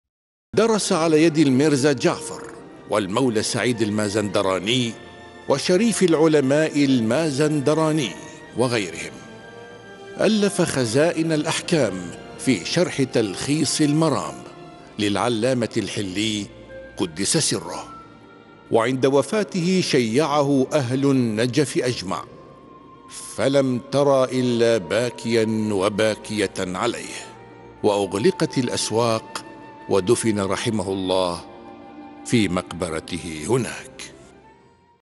مفكرة الأيام - 25 صفر
وفاة الشيخ علي الخليلي
25 صفر 1444 هـ